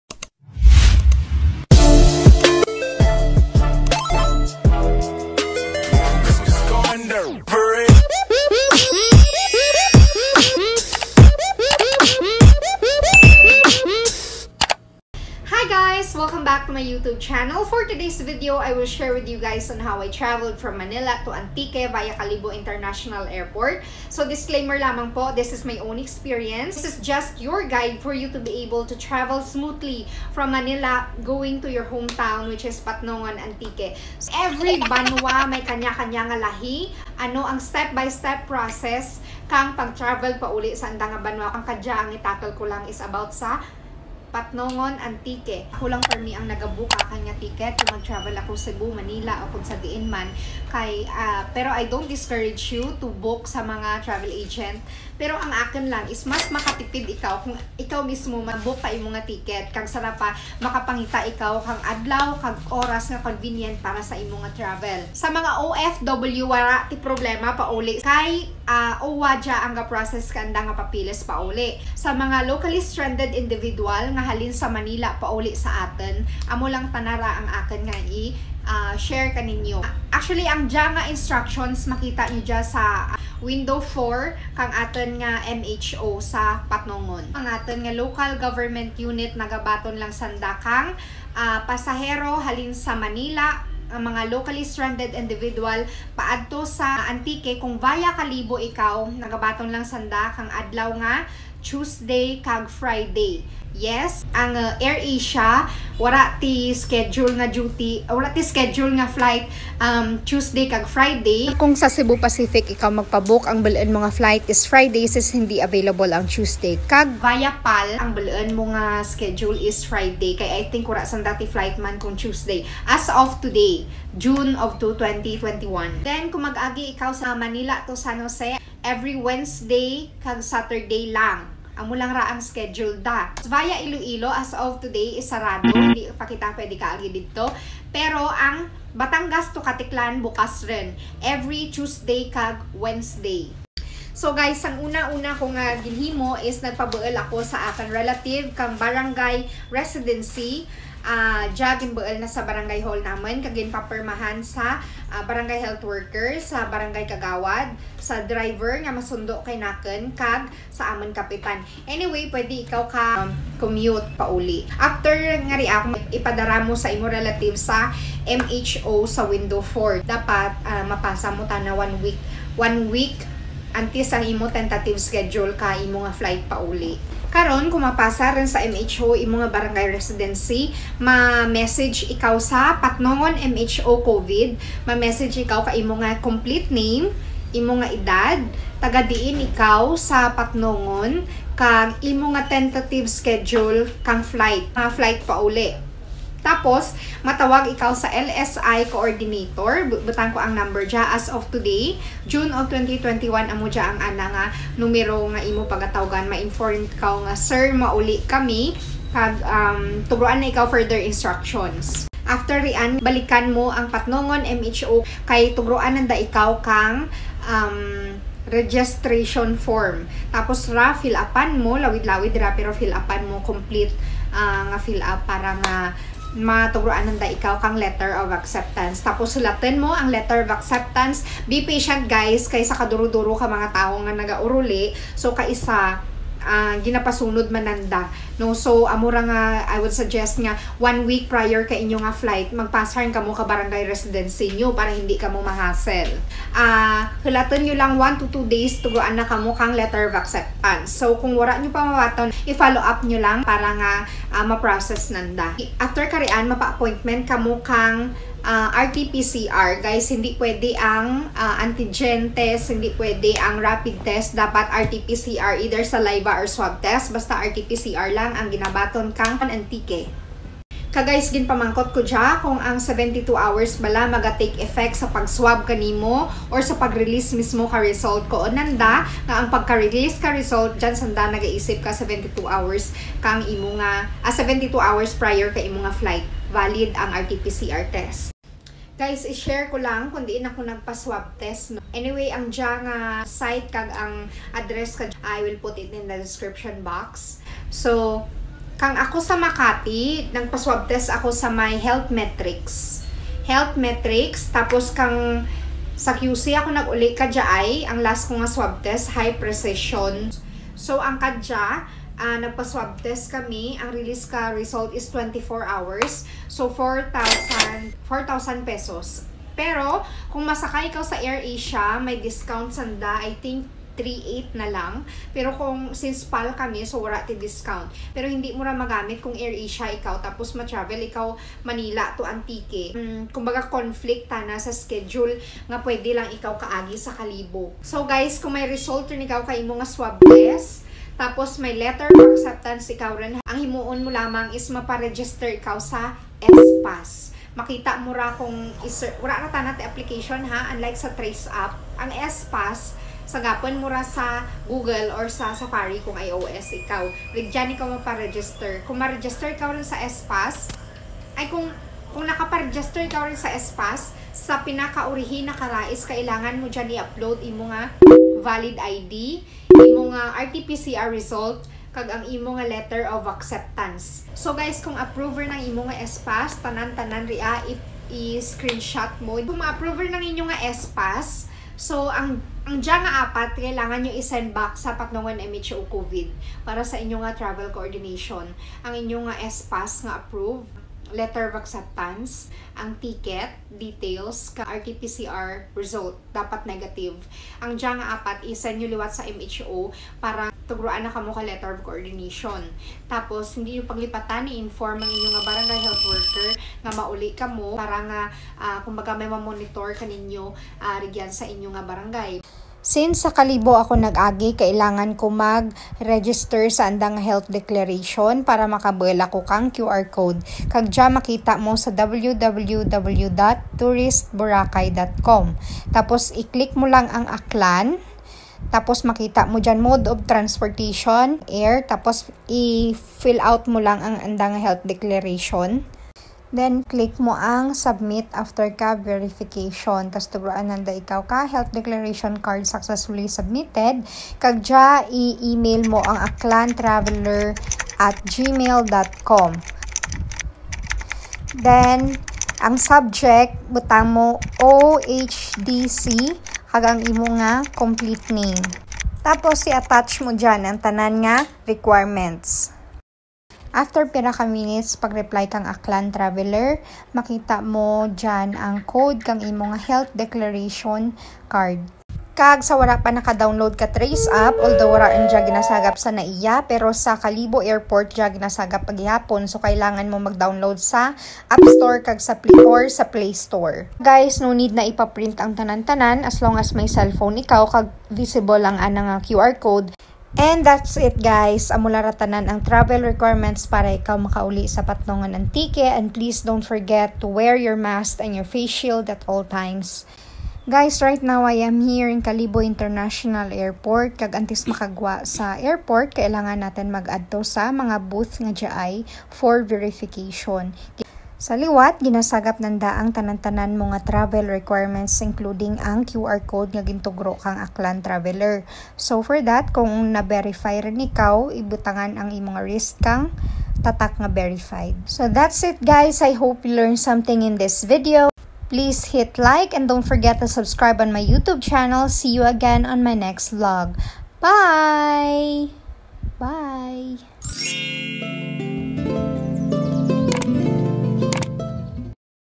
UPDATED: MANILA to ANTIQUE step by step guide in KINARAY-A | jennrezzel
*(MHO I mean RHU) 😂✌🏽
I filmed this video June 2021, the guidelines may change depending on the Executive Order of the Governor of Antique or by your LGU. But I hope you learned something in this video. Here are the site for the Laboratory where I took my RT PCR test:
✅Hi- Precision Diagnostics in NS AMORANTO Branch:
✅ Health Metrics Inc. in MAKATI